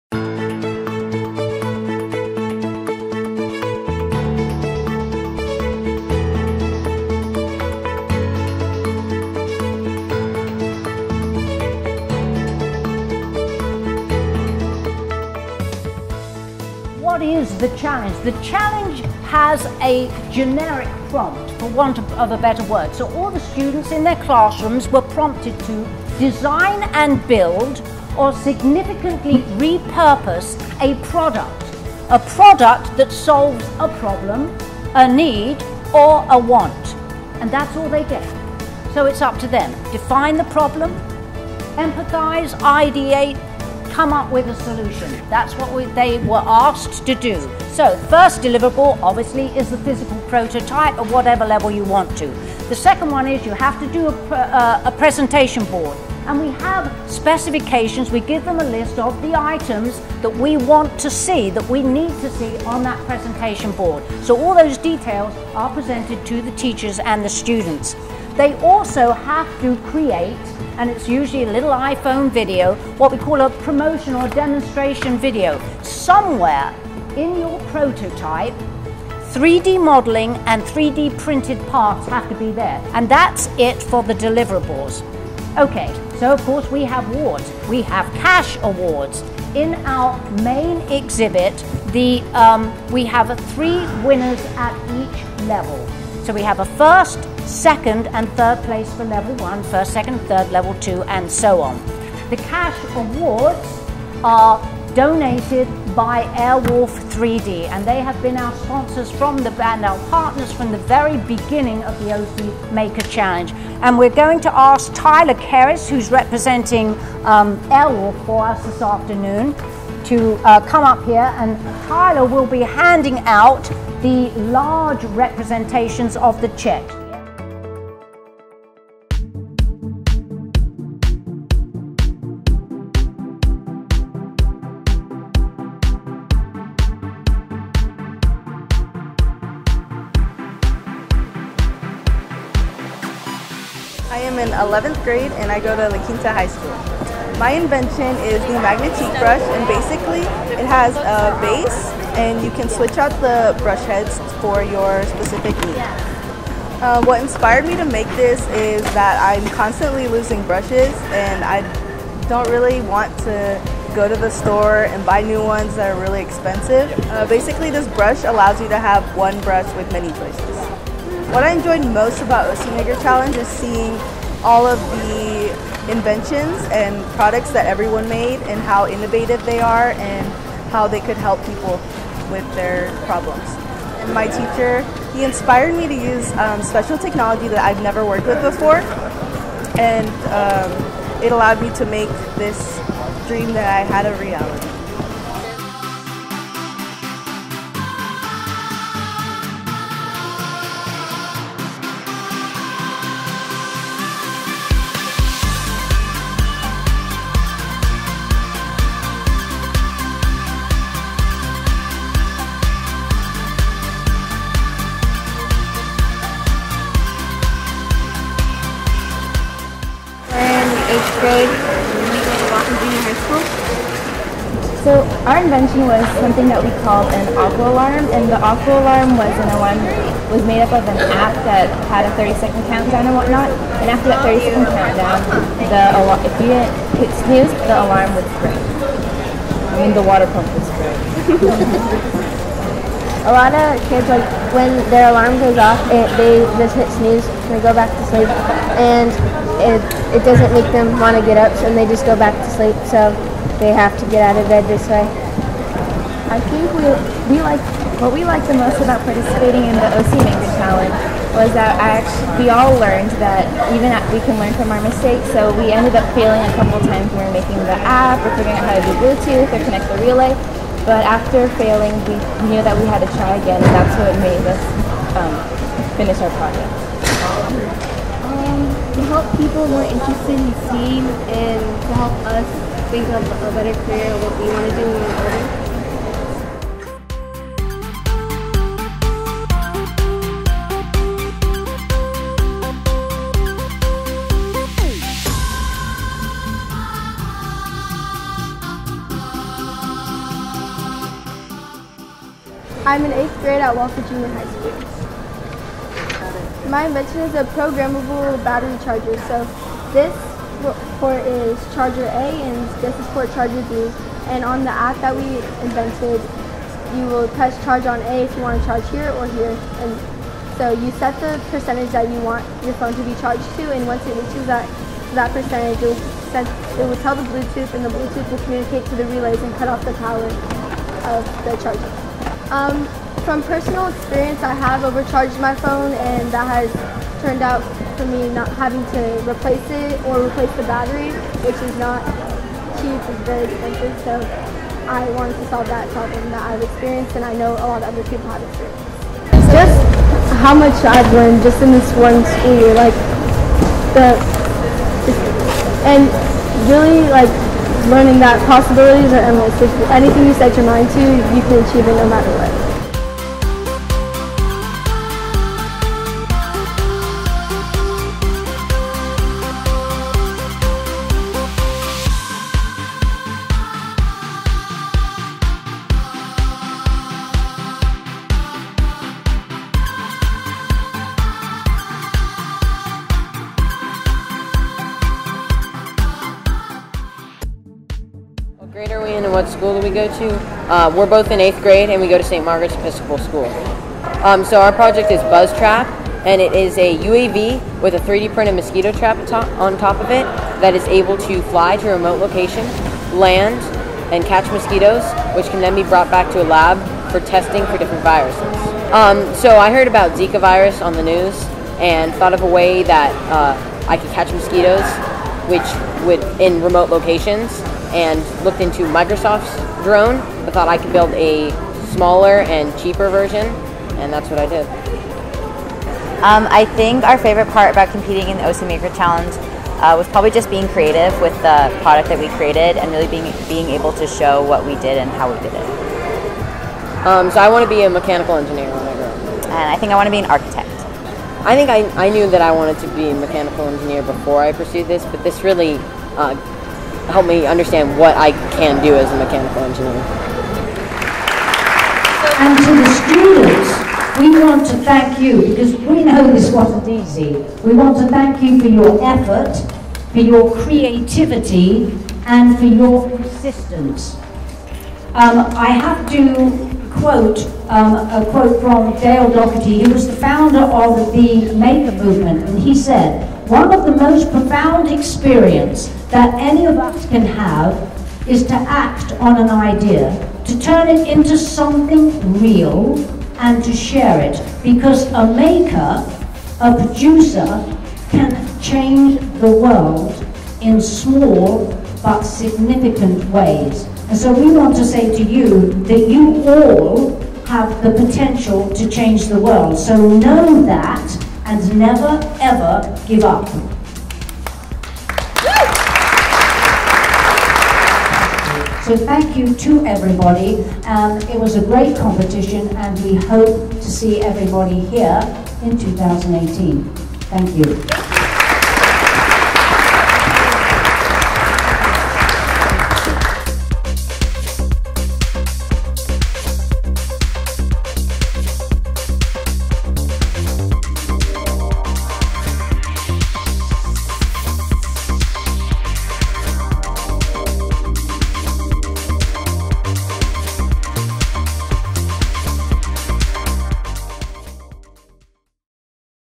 California's Largest STEM and 3D Printing Competition: ocMaker Challenge 2017
What is the oc Maker Challenge?

The ocMaker Challenge is an integrated STEM design competition where students are challenged to "Design and Build (or significantly repurpose) a physical product, that will solve a problem, a need or a want."

Organized and promoted by the Orange County Department of Education's Career Technical Education Partnership and supported by Vital Link, the ocMaker Challenge is designed to ignite students' passion for STEM subjects. Airwolf 3D proudly serves as the event's main industry sponsor/3D printing trainer provider.

To learn more about 3D printing in the classroom, please